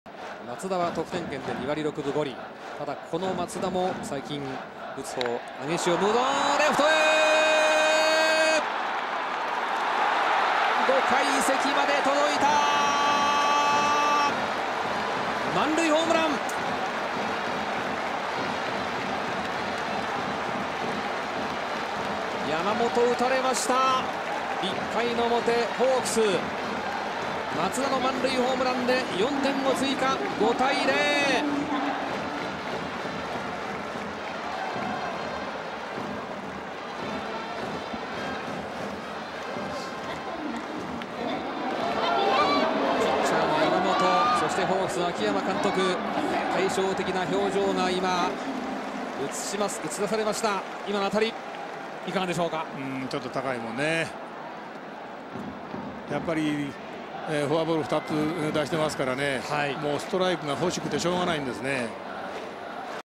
体勢崩されてもエグイ当たり… 松田宣浩の満塁アーチ
昨日も貴重な場面での活躍を見せた福岡ソフトバンク・松田宣浩が絶好調！1アウト満塁の場面で、レフトスタンド5階席まで届く大きな一撃を放った。(2010年5月4日 オリックス・バファローズ対福岡ソフトバンクホークス)